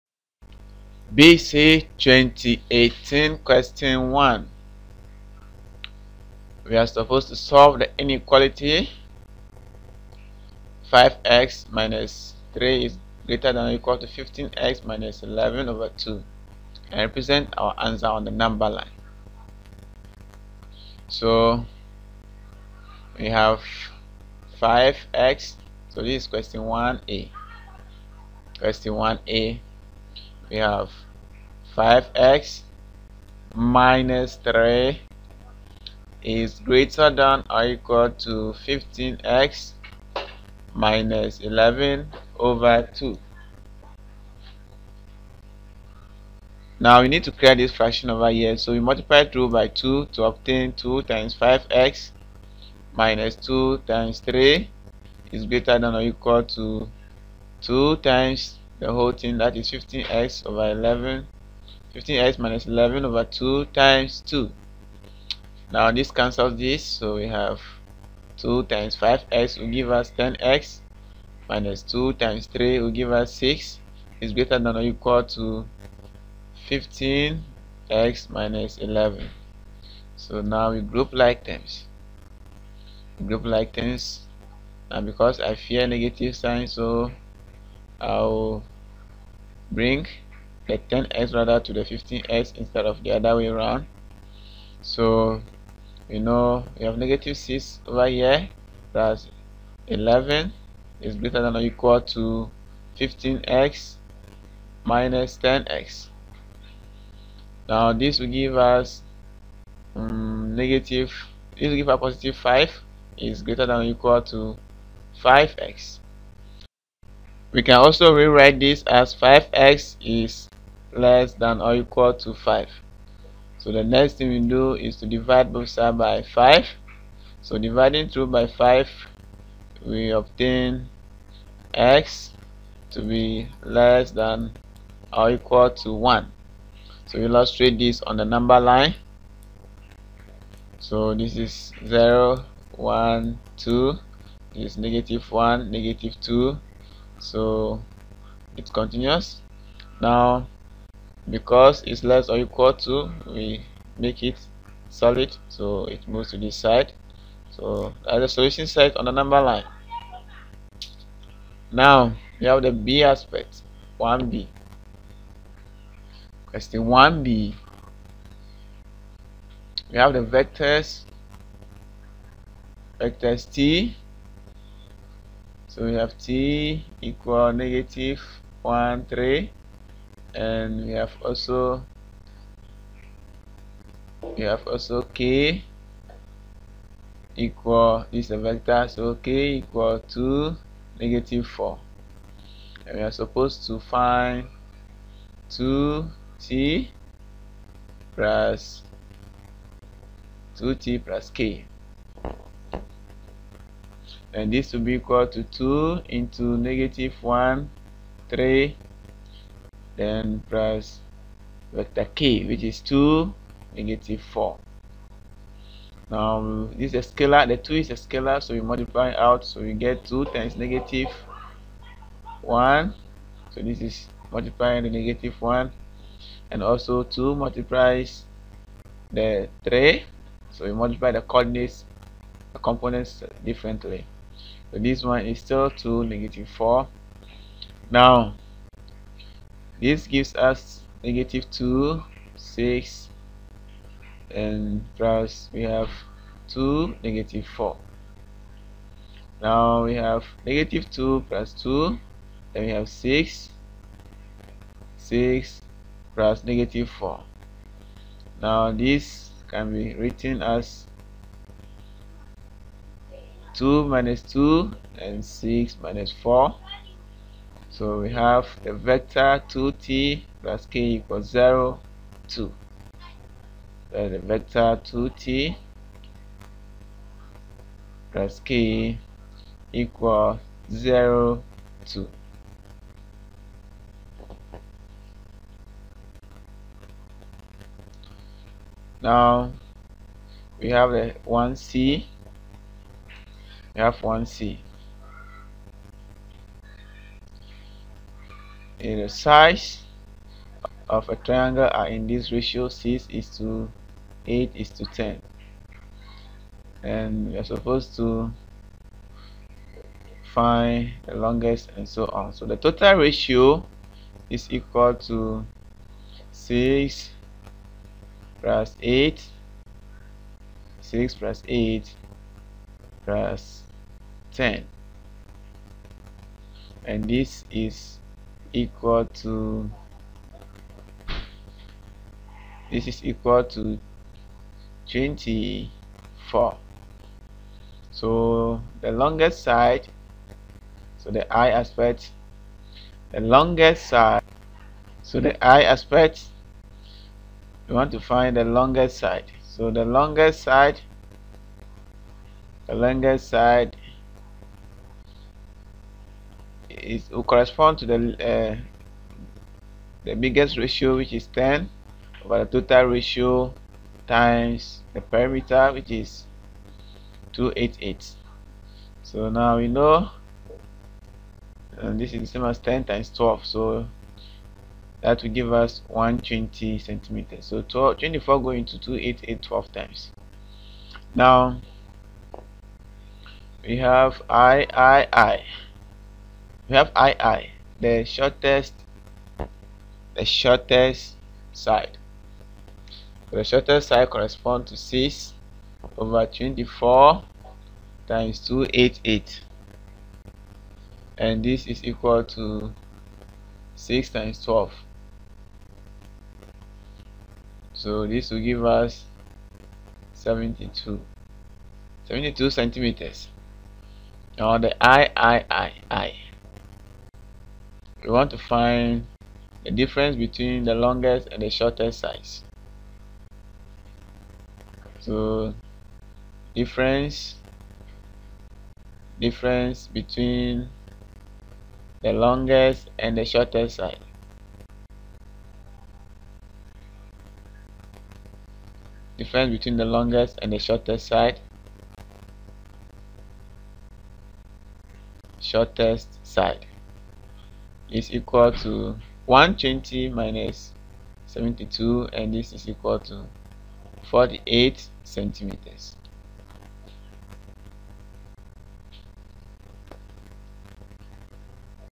BECE 2018 Mathematics Papers 2 Question 1
Here is the solution to BECE 2018 question 1.
The question is solved step by step to help you understand and solve similar BECE mathematics questions.